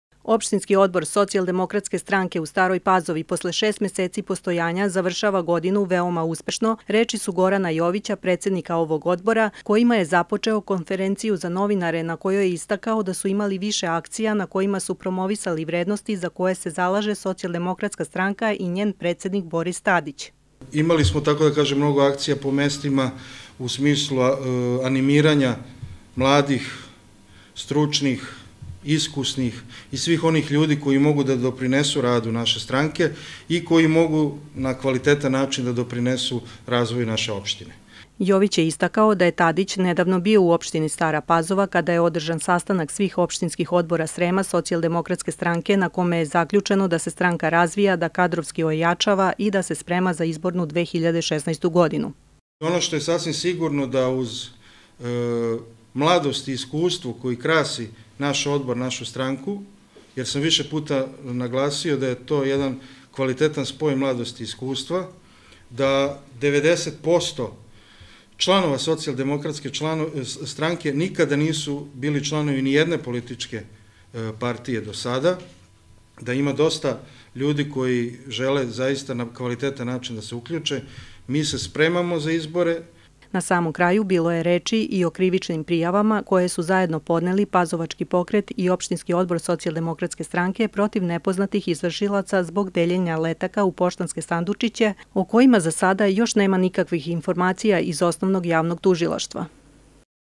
Konferencija SDS
U prostorijama OOSDS u MZ Stara Pazova održana poslednja ovogodišnja konferencija za novinare, na kojoj se predstavnicima medija obratio Goran Jović predsednik OOSDS Stara Pazova, a prisustvovali su i Dr Milinka Maćerak, potpredsednica ovog odbora i Boris Babik, poverenik Mesnog odbora SDS Stara Pazova.